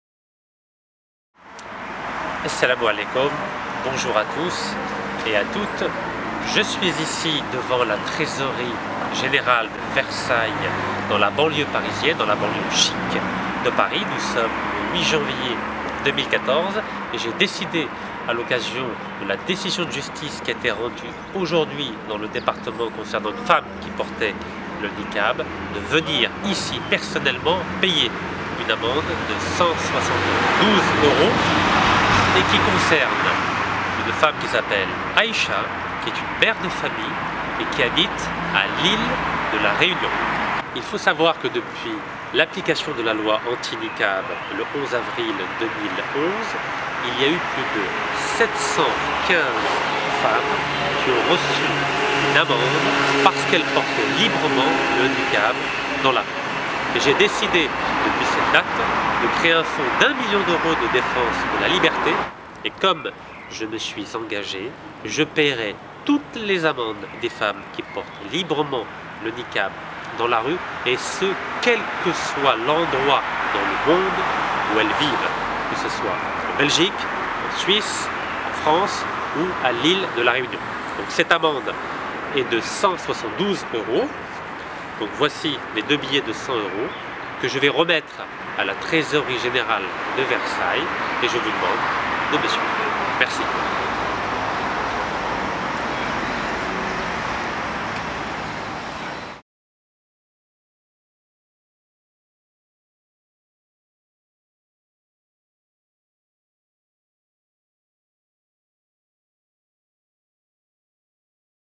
Rachid Nekkaz Paiement Amendes Femme Niqab
Rachid Nekkaz candidat aux élections présidentielles algériennes  2014 continue de payer les amendes des femmes en Niqab. Rachid Nekkaz a payé 716 amendes pour 487 musulmanes algériennes, françaises et belges pour garantir à ces femmes la liberté de porter l'habit de leur choix.